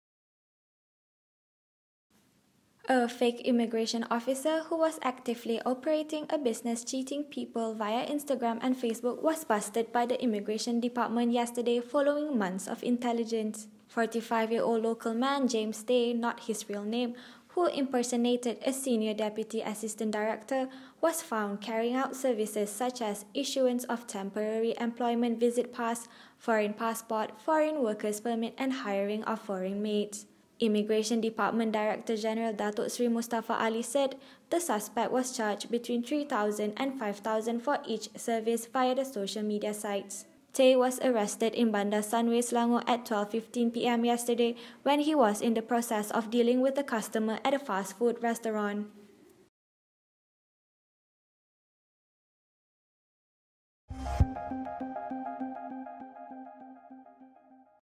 Immigration Dept detains bogus officer for cheating on Facebook, Instagram
PUTRAJAYA: A fake immigration officer who was actively operating a business cheating people via Instagram and Facebook was busted by the Immigration Department yesterday following months of intelligence.